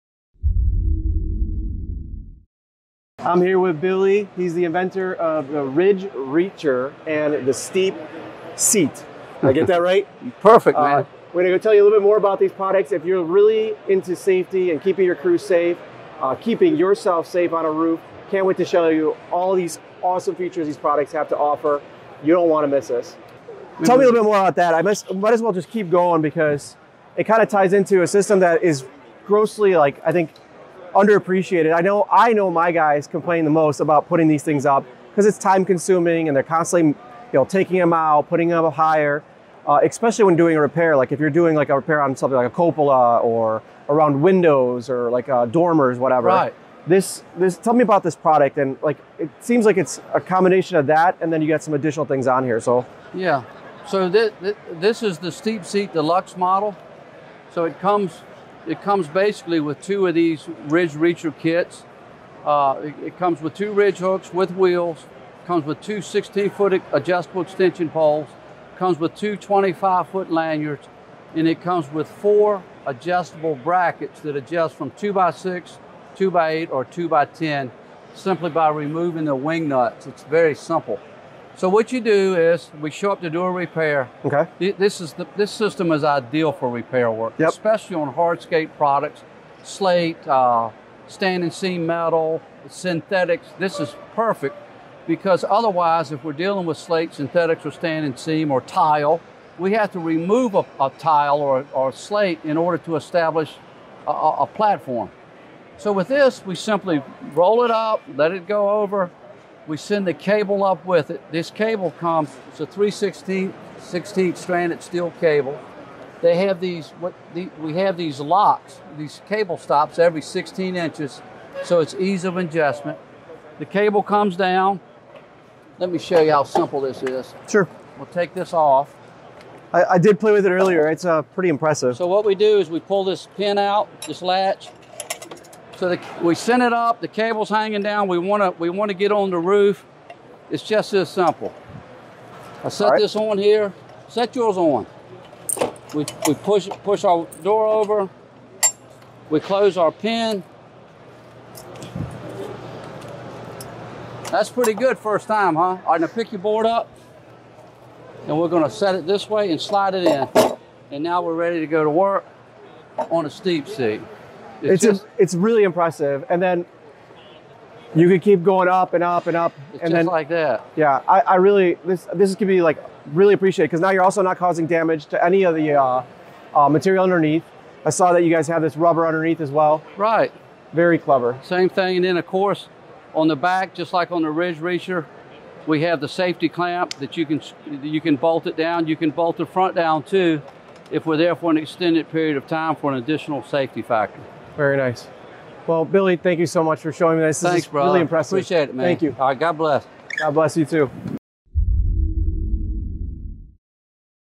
The SteepSeat | Revived Exteriors - Safest Way to Do Repairs on Roof!
Let's learn about the Steep Seat made by William Kain with KainBuilt and how it can making your roofing jobs easier!

Steep Seat allows you to complete repairs of the roof without the high risk of falling. In addition, you don't have to nail anything to the roof to provided extra safety when working on the roof repair or replacement.

Insights:
0:02 Intro
0:50 What it comes with
1:20 What it's made of
1:45 How to use it